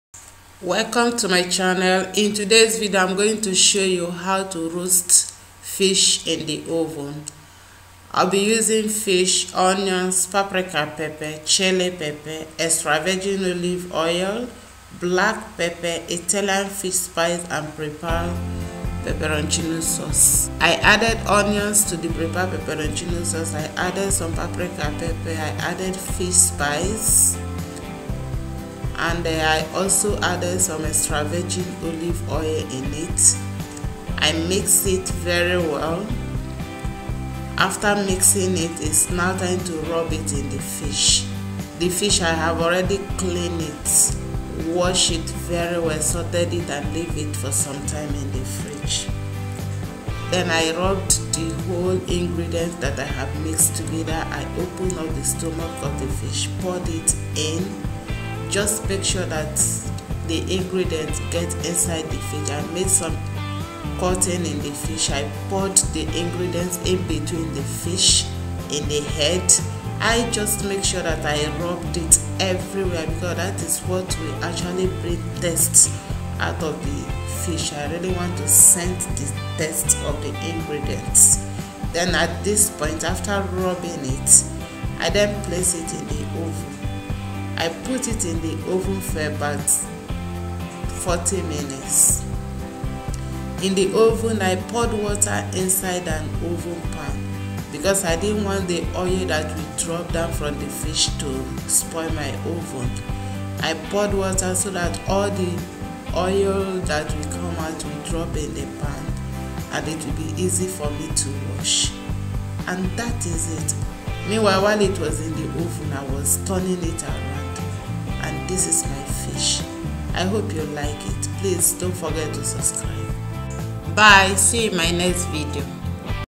How to make oven grilled fish ( NIGERIAN OVEN ROASTED FISH)
In today's video am going to show you how to roast fish in the oven  Nigerian oven grilled fish I will be using the following ingredients,
 to grill my fish in the oven(Nigerian oven grilled fish) fish, onions, paprika pepper, chili pepper, Extra virgin olive oil, black pepper,Italian fish spice and peperroncino saurce